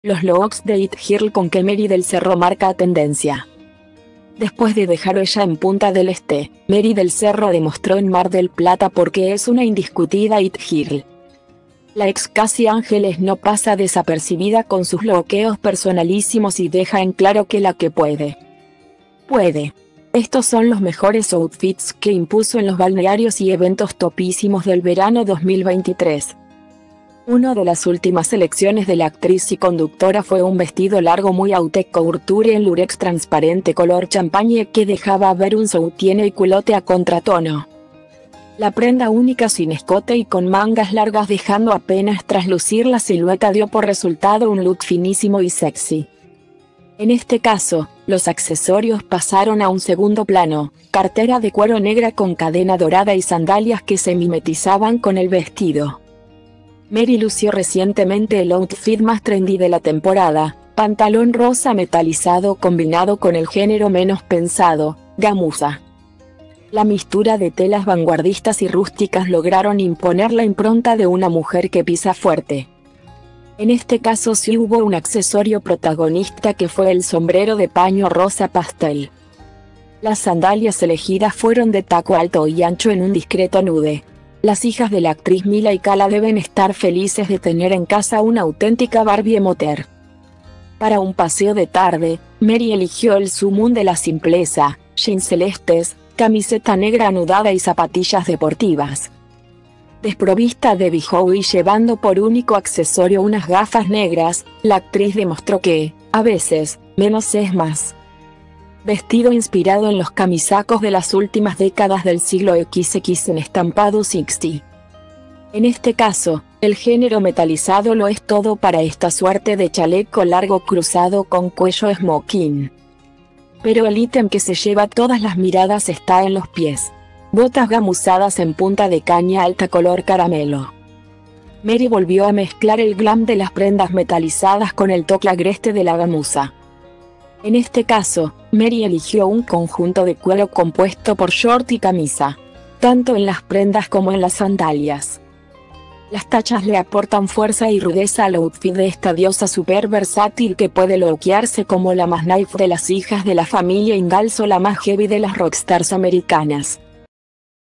Los looks de "it girl" con que Mery del Cerro marca tendencia
Después de dejar huella en Punta del Este, Mery del Cerro demostró en Mar del Plata por qué es una indiscutida “it girl”.La ex “Casi ángeles” no pasa desapercibida con sus lookeos personalísimos y deja en claro que “la que puede,puede”. Estos son los mejores outfits que impuso en los balnearios y eventos topísimos del verano 2023.